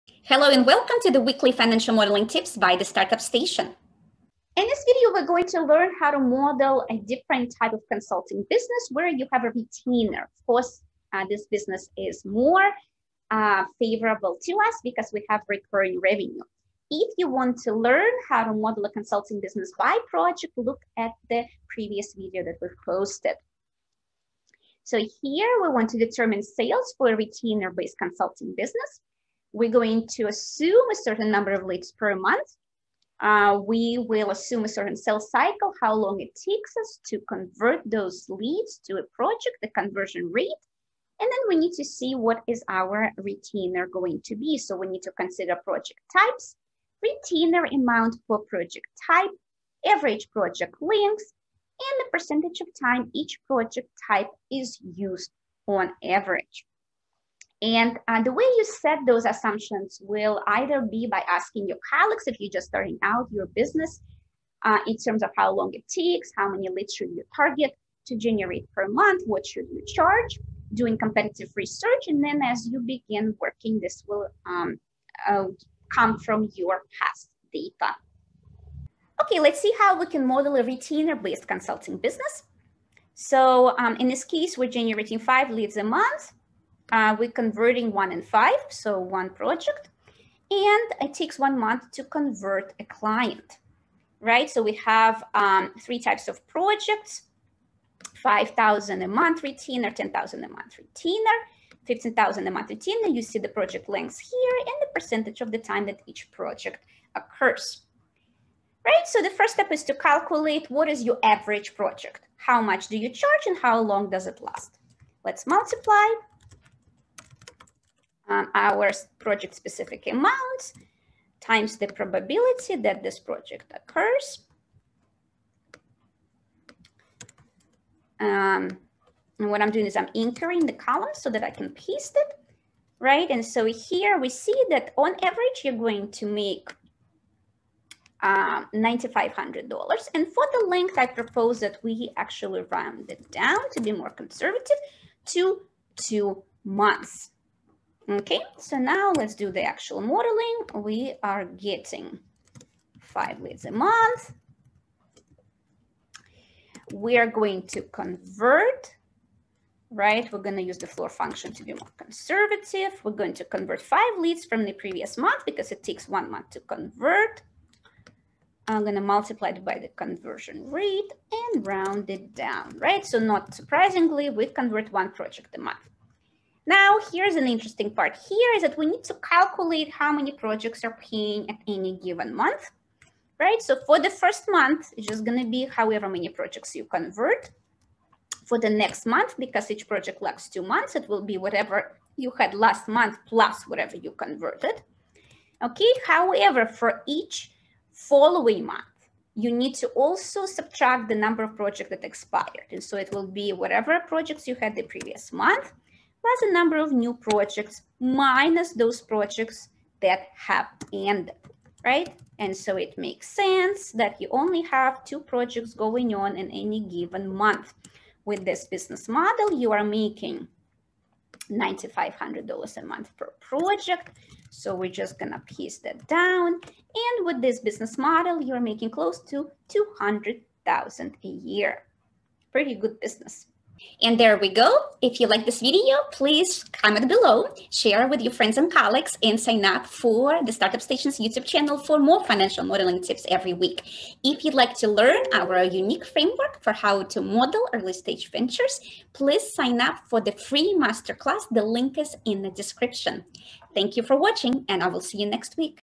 How to Calculate Revenues for Retainer-based Consulting - Financial Modeling Tip
In this video, The Startup Station's founder and President, Victoria Yampolsky, discusses how to calculate revenues for a retainer-based consulting business.

We cover how to formulate assumptions and how to model each scenario in Excel. This is applicable to any company, including a pre-revenue startup.